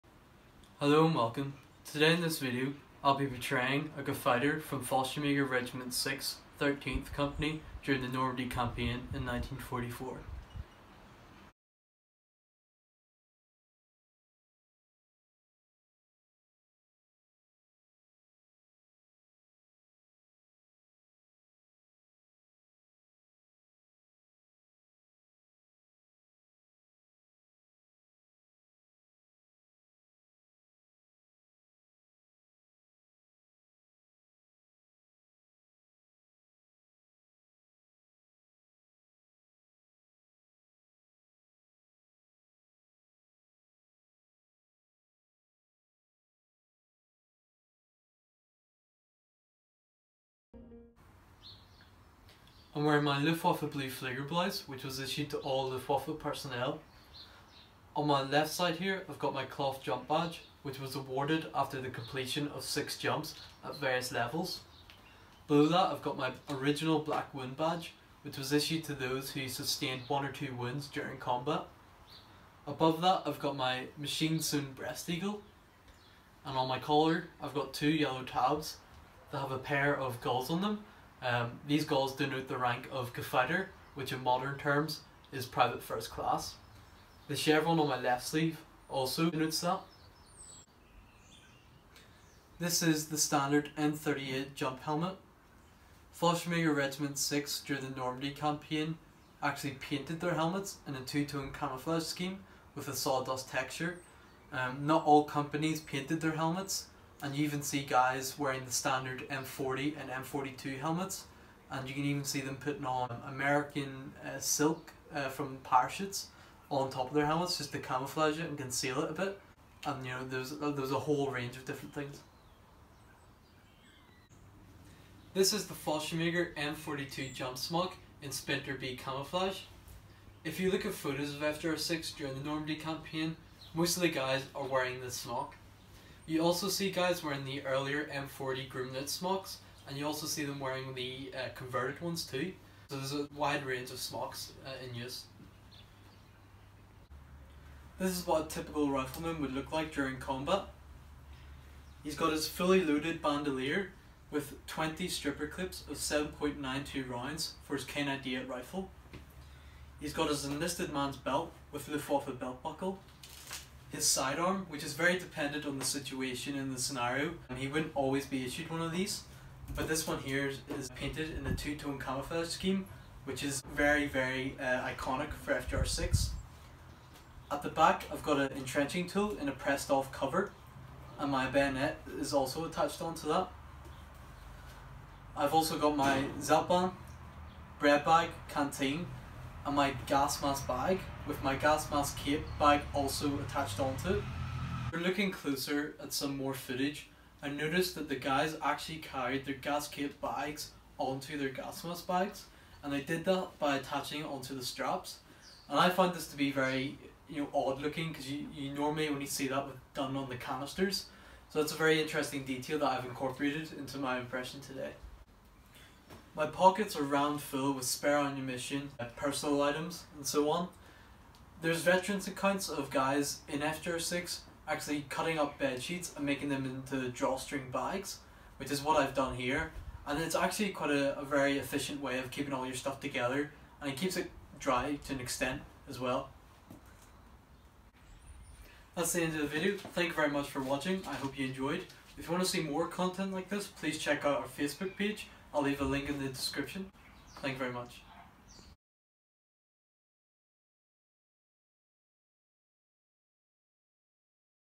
WW2 Fallschirmjäger Regiment 6, 13.Kompanie Impression Study.
Hello guys here’s another video!
FJR6 during the Normandy campaign has always been a huge interest to me and I’ve always wanted to capture the look of unit to the best of my ability!